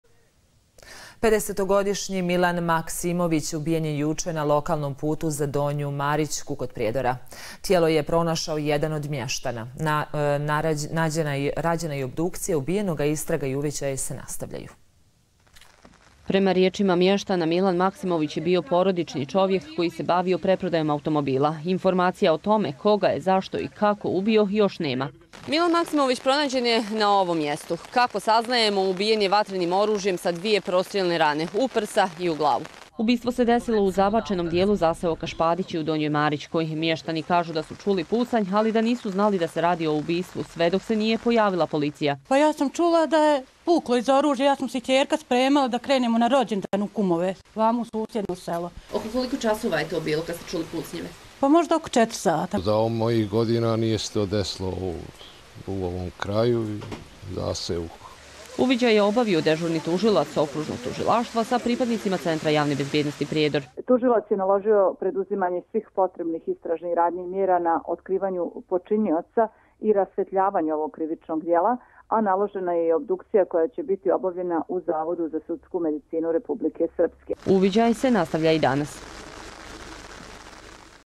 Prijedor- Ubijen Milan Maksimović
Milan Maksimović (50) ubijen je danas u zaseoku Špagići kod Prijedora, rečeno je u banjalučkom Okružnom tužilaštvu.